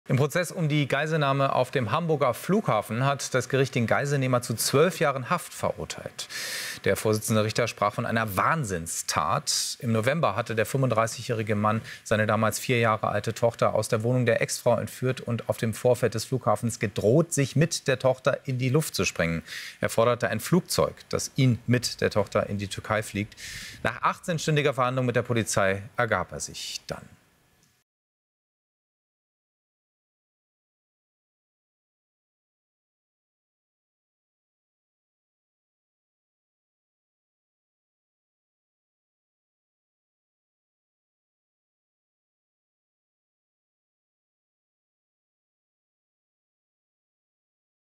HAMBURG: Nahm seine eigene Tochter als Geisel! Zwölf Jahre Haft für Flughafen-Entführer!
Beinahe acht Monate nach der dramatischen Entführung seiner vierjährigen Tochter am Hamburger Flughafen wurde ein 35-jähriger Mann zu zwölf Jahren Haft verurteilt. Das Landgericht Hamburg sprach ihn unter anderem der Geiselnahme schuldig. Der Vorsitzende Richter betonte in der Urteilsbegründung die traumatischen Folgen für das Kind und die Mutter. Die Einstellungen des Mannes seien schwer erträglich.

Der Hintergrund der Tat war ein Sorgerechtsstreit. Am Abend des 4. Novembers entführte der Beschuldigte seine Tochter aus der Wohnung der getrennt von ihm lebenden Mutter in Stade, Niedersachsen. Er fuhr mit dem Kind zum Hamburger Flughafen, durchbrach dort ein Tor und gelangte mit einer Pistole und Brandsätzen bewaffnet bis auf das Rollfeld. Der Mann rief den Notruf und behauptete, Bomben bei sich zu haben. Er warf brennende Molotowcocktails aus dem Auto und schoss in die Luft, was eine Sperrung des Flughafens und einen stundenlangen Einsatz von Spezialkräften zur Folge hatte. Nach 18 Stunden gab er auf und übergab seine Tochter unverletzt der Polizei. Bei seiner Festnahme hatte er auch die Attrappe eines Sprengstoffgürtels und ein Messer bei sich.

Der Beschuldigte wollte eine ungehinderte Ausreise mit seiner Tochter in die Türkei erzwingen. Bereits ein Jahr zuvor hatte er das Kind unrechtmäßig für mehrere Monate in die Türkei gebracht. Die Mutter konnte das Mädchen damals zurückholen, und der Vater wurde zu einer Geldstrafe verurteilt. Nun wurde der Mann auch wegen Entziehung Minderjähriger und Verstößen gegen Waffengesetze verurteilt. Die Kammer wertete strafschärfend die massiven Folgen der Tat für Mutter und Kind sowie die große kriminelle Energie des Angeklagten und den hohen Schaden für den Hamburger Flughafen, der mehrere Millionen Euro betrug.

Obwohl der Mann die Tat vor Gericht gestand, relativierte er gleichzeitig ihre Schwere und behauptete, im Recht zu sein.